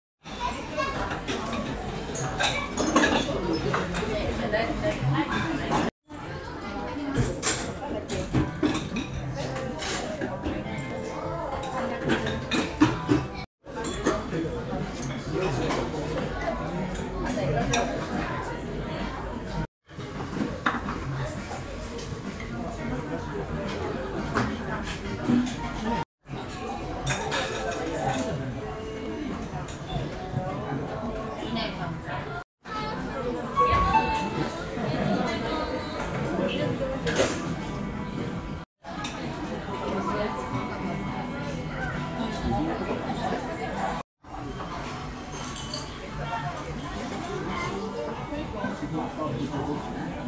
2022年11月12日にはま寿司で食べたお寿司の様子まとめ！
2022年11月12日にはま寿司で食べたお寿司の様子をまとめた動画を作成しました！
今回は炙りとろ銀鱈、生しらす(日本近海産)、真鱈白子軍艦、宮城県気仙沼産とろかつお軍艦、えび天、オクラ天、カリカリポテト(のり塩味)を実食。

炙りとろ銀鱈　　0:00
生しらす(日本近海産)　　0:06
真鱈白子軍艦　　0:13
宮城県気仙沼産とろかつお軍艦　　0:20
えび天　　0:26
オクラ天　　0:32
カリカリポテト(のり塩味)　　0:39